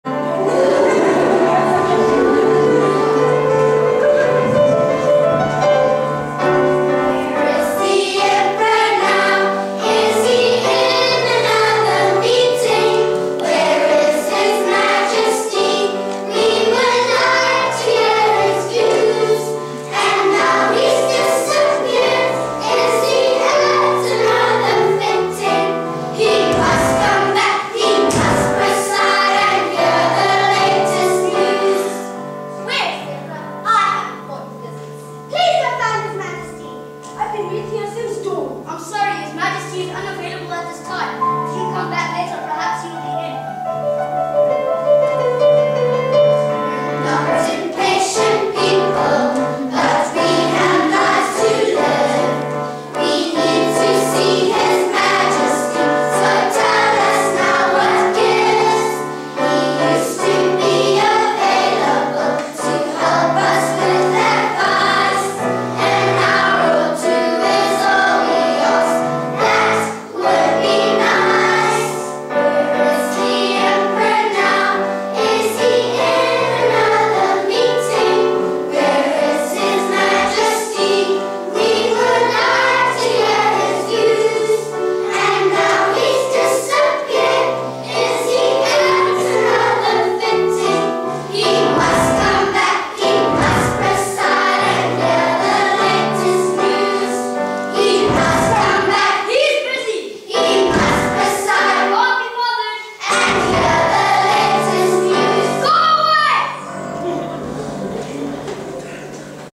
Clifton Prep School Grade 3 Play - Scene 5
Scene 5 from the grade 3 school play: The Emperors New Clothes